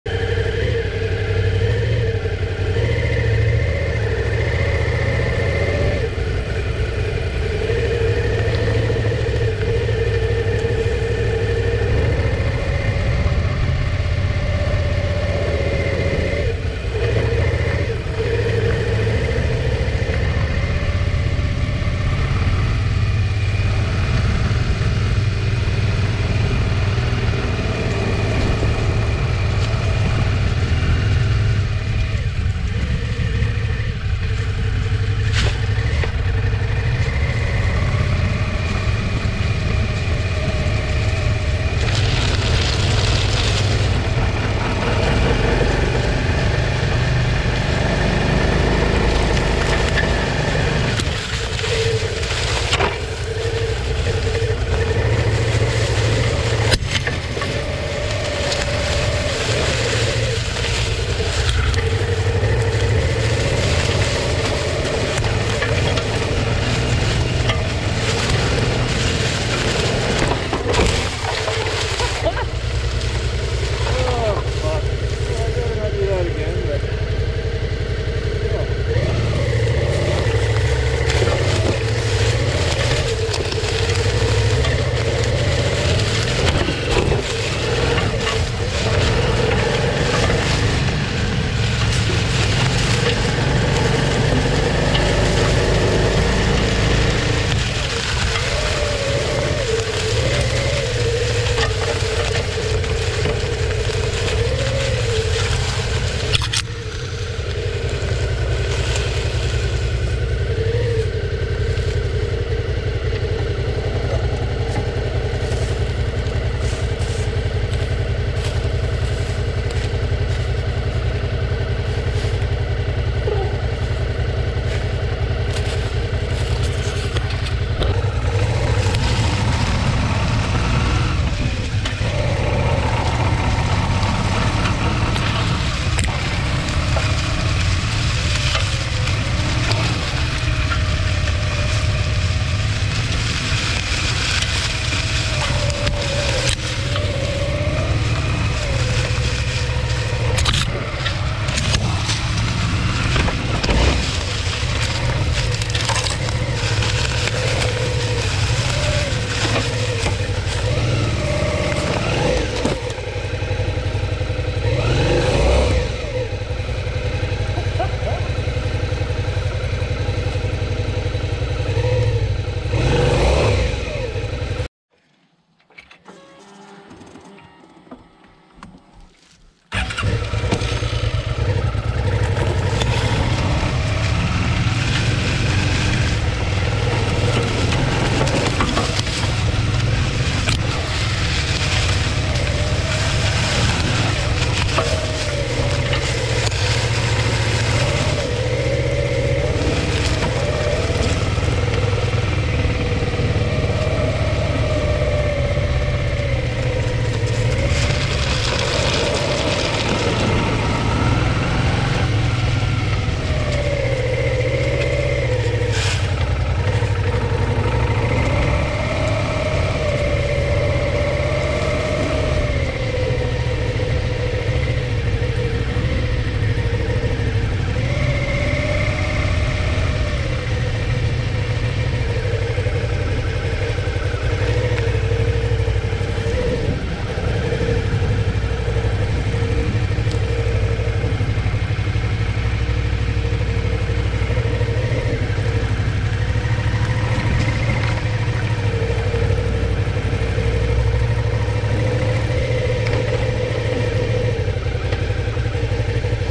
Suspension Camera Angles Testing
In preparation for the comparative review of the 2017 V-Strom 650XT stock suspension VS Cogent DDC & Straight Rate spring upgrade. Let me know what you think!